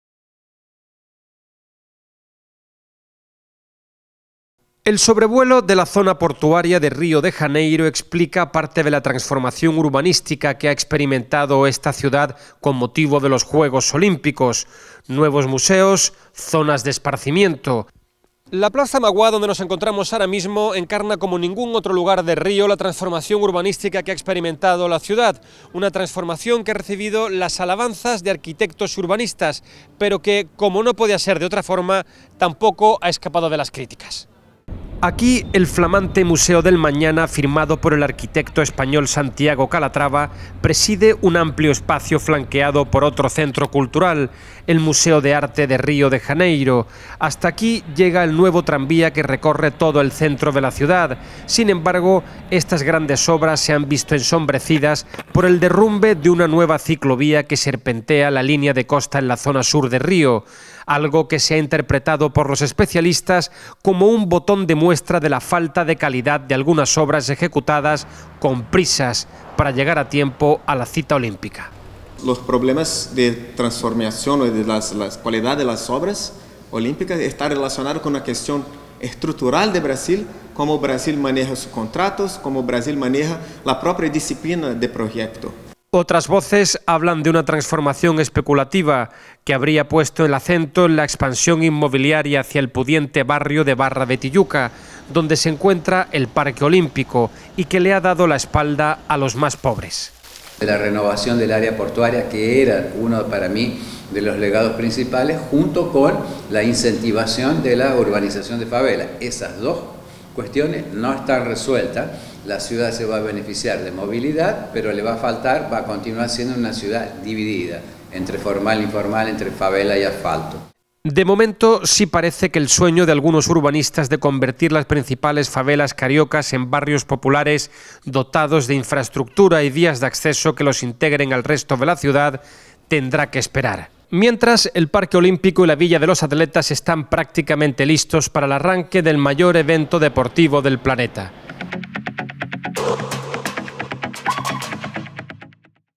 Río de Janeiro y su transformación urbanística para los Juegos Olímpicos
Especialistas cuestionan la falta de calidad de algunas obras ejecutadas con prisa. Compartimos el informe de Francho Barón para Olímpicos.